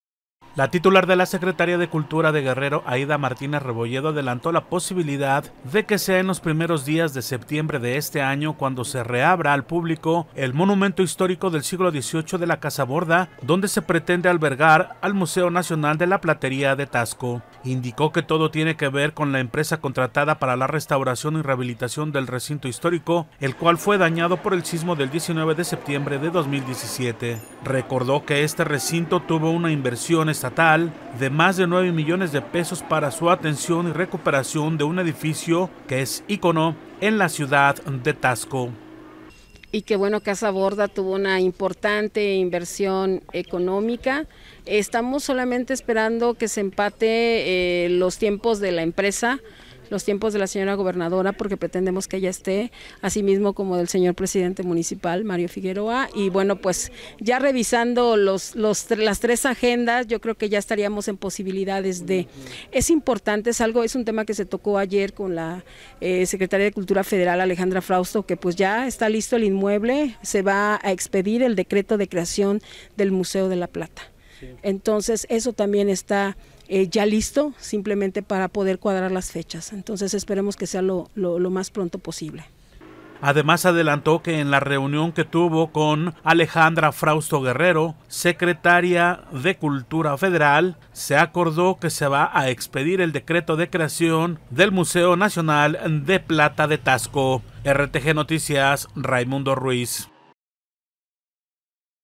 RTGnoticias | Casa borda será reabierta en septiembre: SECULTURA - 30 de agosto de 2023
La titular de la Secretaría de Cultural de Guerrero (Secultura), Aida Martínez Rebolledo adelantó la posibilidad de que sea en los primeros días de septiembre, cuando se reabra al público el monumento histórico del siglo XVIII de la Casa Borda, donde se pretende albergar al Museo Nacional de la Plata de Taxco.